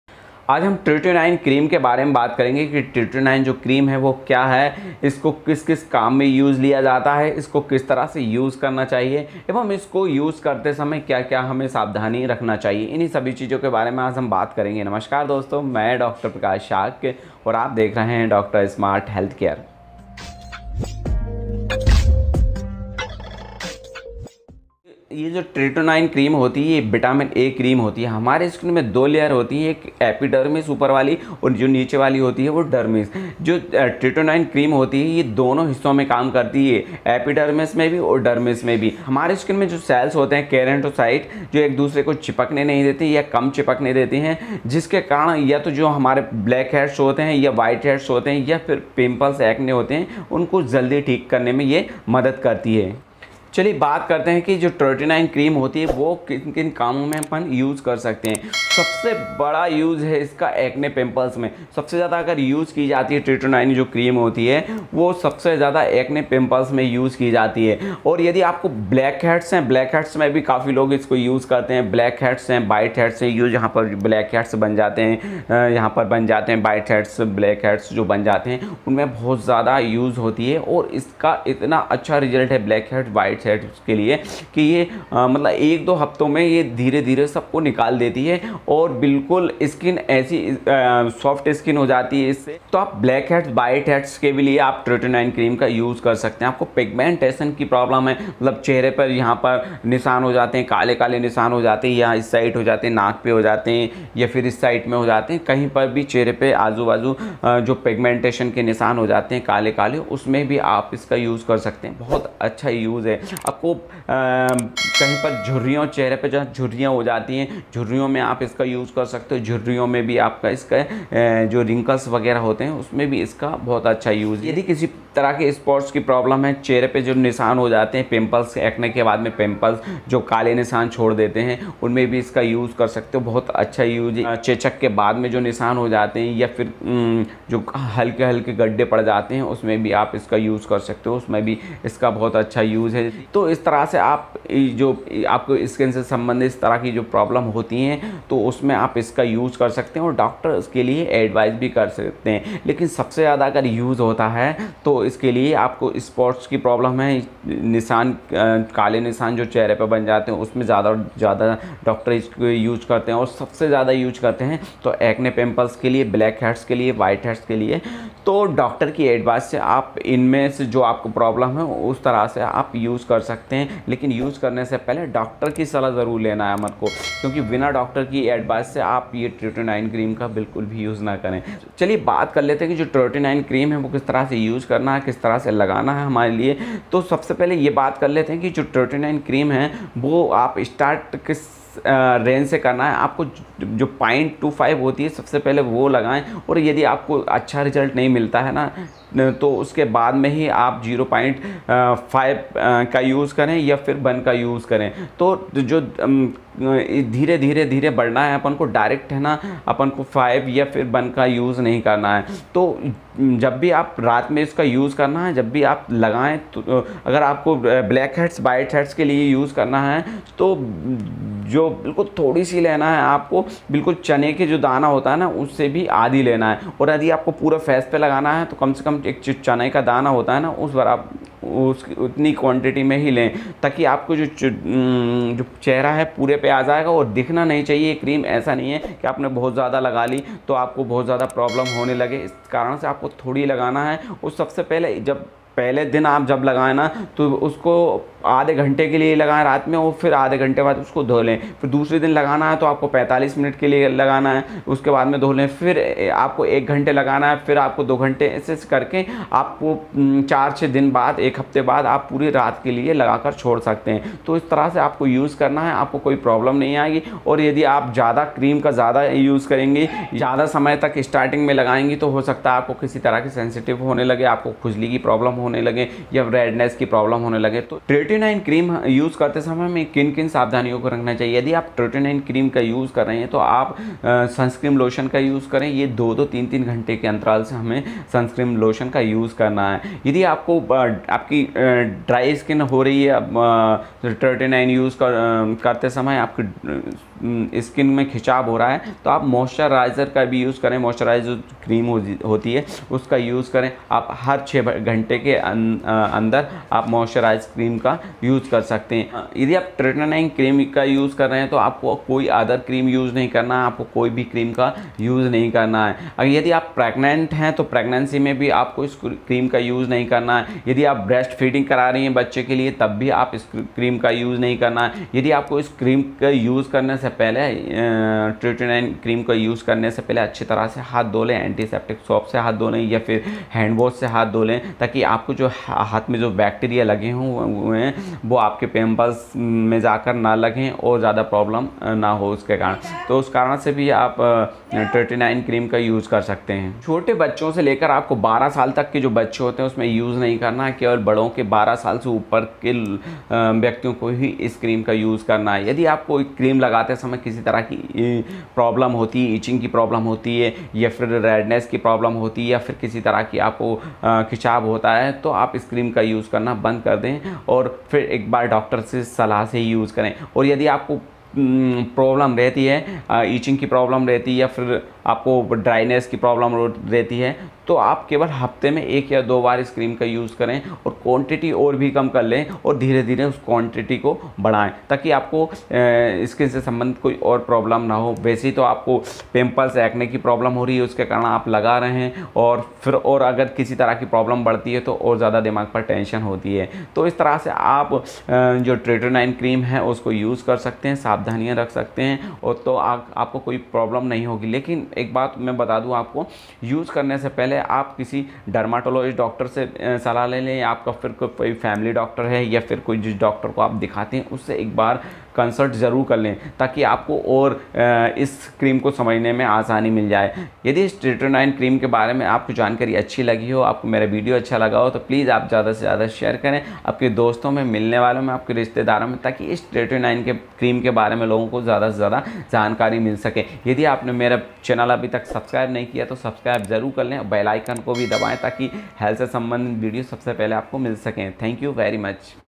Tretinoin cream, क्या ट्रेटिनोइन क्रीम आपके पिंपल्स को ठीक कर सकती हैं ?
इस वीडियो में Tretinoin cream के बारे में जानकारी दी गई है कि tretinoin cream क्या है इसको किस तरह से इस्तेमाल करें एवं इसके साइड इफेक्ट क्या है इस वीडियो को देखकर आप पूरी जानकारी प्राप्त कर सकते हैं !